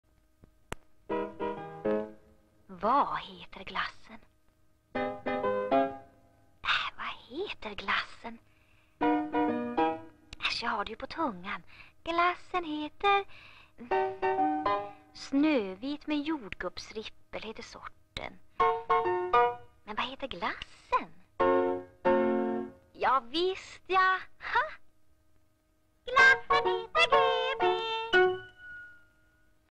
Reklam för GB Snövit (1967)
Reklam för GB Snövit.

Medverkande: Lena Granhagen

Film från år 1967.